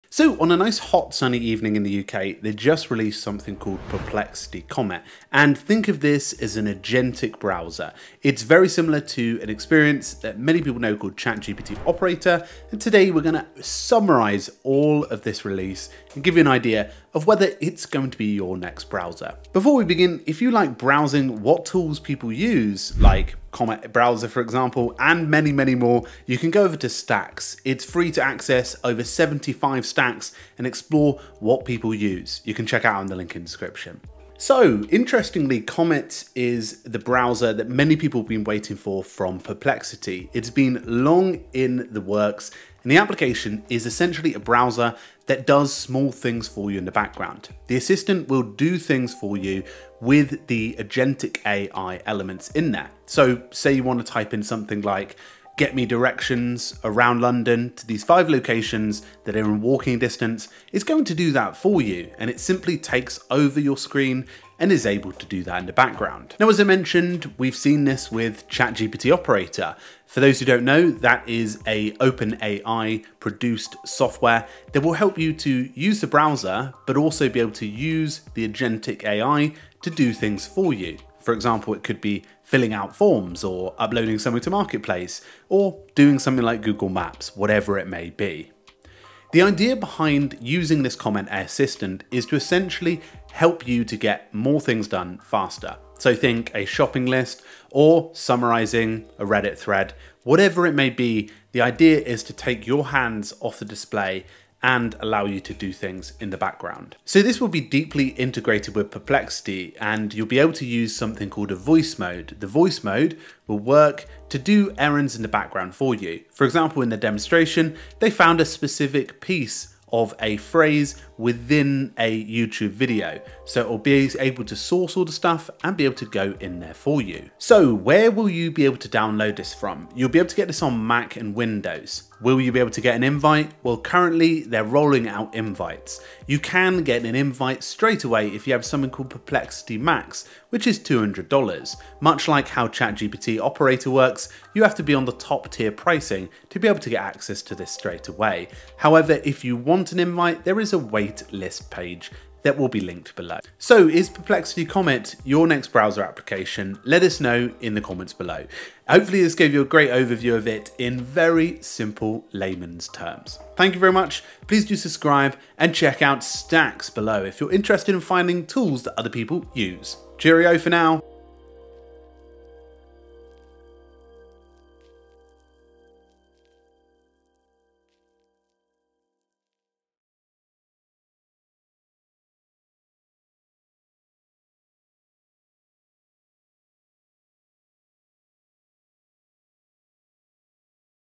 Perplexity Comet Browser is here!
Perplexity Comet is a new browser app and we explain it easily in 3 minutes.

Welcome to The Shortcut AI. The latest in AI in under 2 minutes. Explained simply for you to get the concept, learn the skills & get the AI tools for the job.